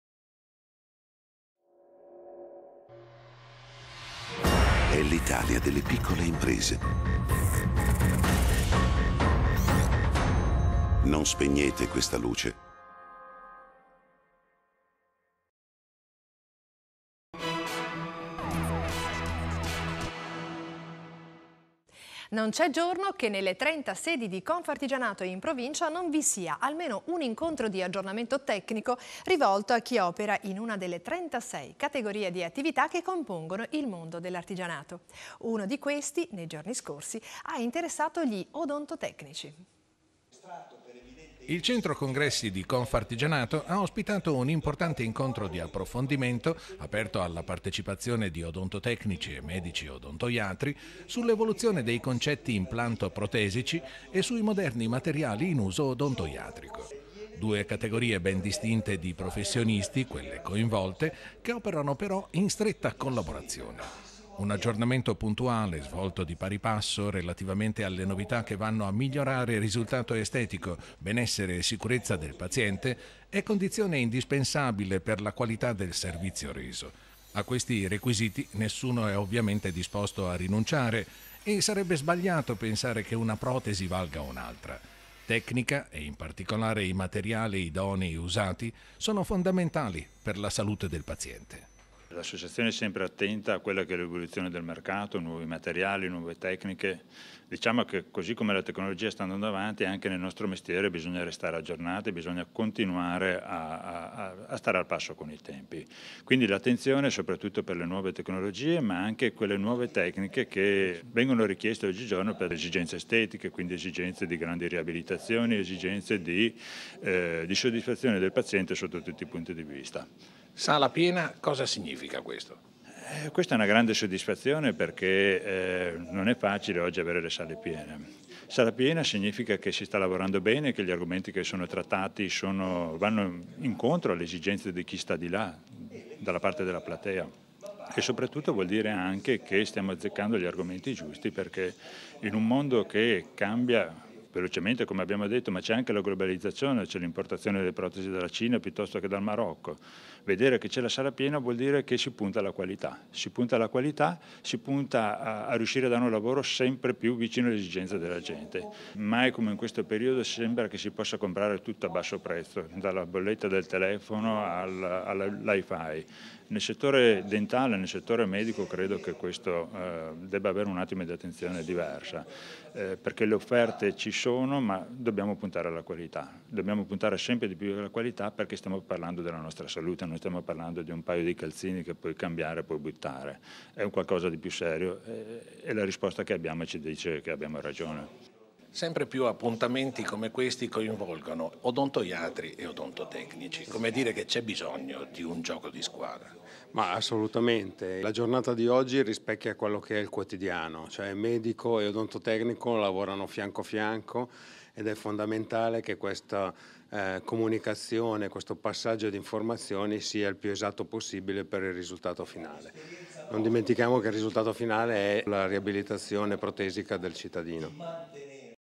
Gioco di squadra tra odontoiatri e odontotecnici per la migliore riabilitazione protesica
Confartigianato ha ospitato un affollato incontro di approfondimento, rivolto a odontotecnici e medici odontoiatri, sull’evoluzione dei concetti implantoprotesici. Due categorie di professionisti che operano in stretta collaborazione per garantire risultati estetici ottimali e massima sicurezza degli utenti.